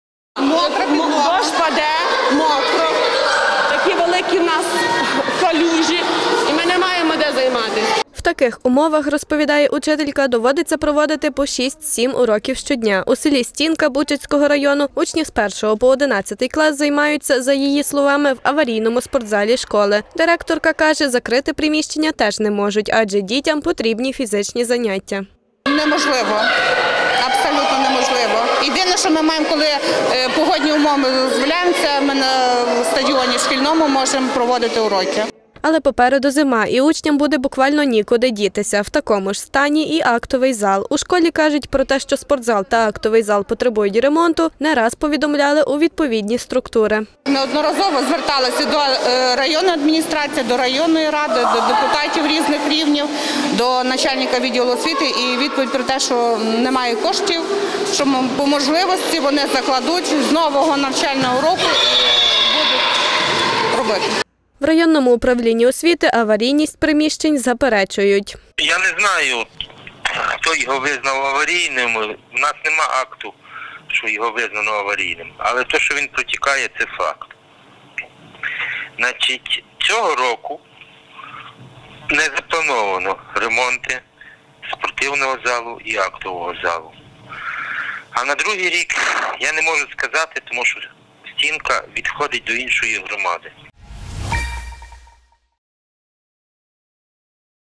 «Аварійна фізкультура»: у селі Стінка школярі займаються в небезпечних умовах
У селі Стінка, Бучацького району, приміщення спортзалу та актового залу школи потребують капітального ремонту.  Попри це,  заняття та уроки там продовжують проводити. Наші кореспонденти побували у школі та розкажуть детальніше про ситуацію.